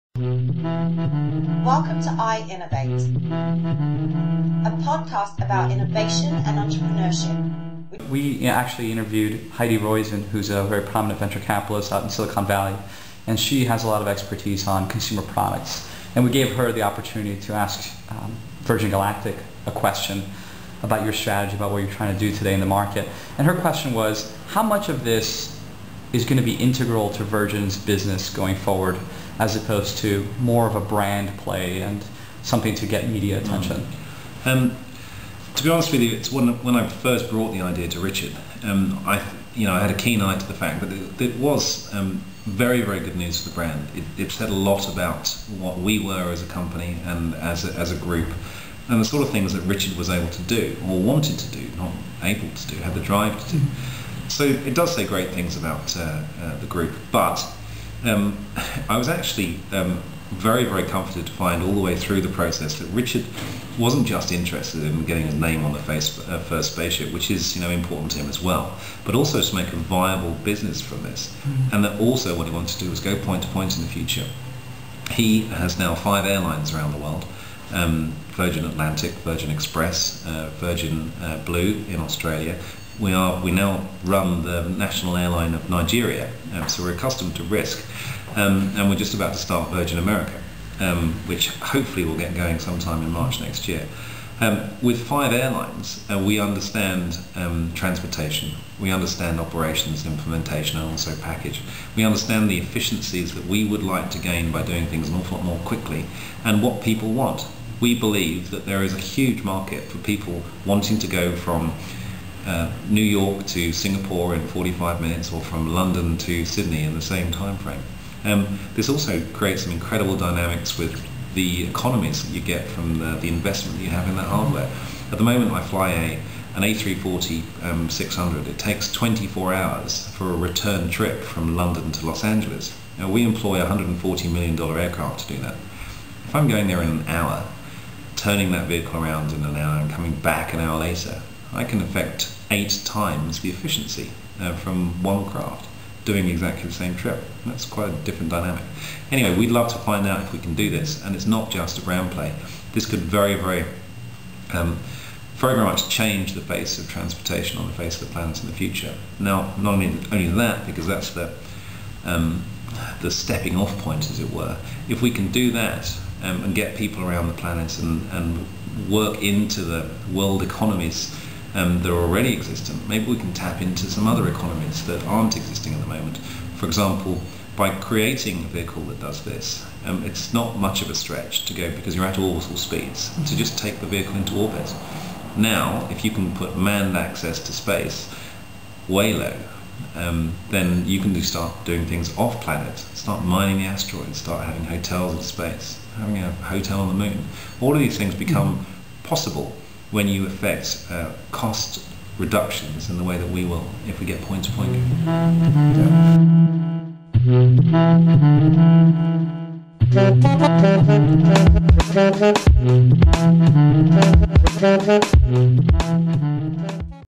Alex Tai of Virgin Galactic Responds to Heidi Roizen
Alex Tai, VP of Operations of Virgin Galactic and Richard Branson's personal friend responds to Heidi Roizen's (Managing Director of Mobius Venture Capital) question about what role Virgin Galactic will play in Virgin's future (the answer: potentially a whole lot!). For more of this iinnovate interview, check out